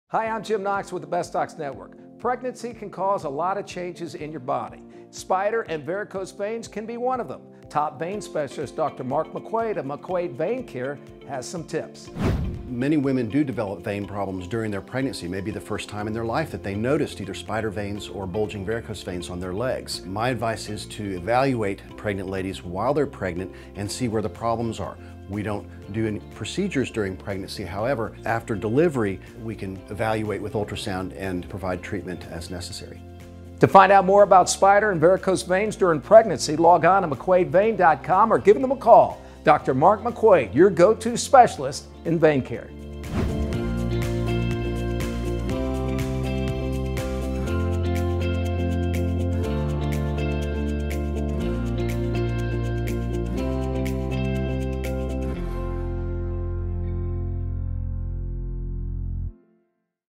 Watch: The Shocking Truth About Pregnancy & Your Veins - What Every Expectant Mother Needs to Know!
In this eye-opening video, we delve into the often-overlooked aspect of pregnancy: its profound impact on your veins. Join us as we uncover the startling realities of how pregnancy can affect your vascular health, from spider veins to varicose veins and beyond. Expert insights, real-life stories, and essential tips await every expectant mother and anyone curious about the hidden challenges of pregnancy. Don't miss out on this must-watch exploration of the veiny side of pregnancy!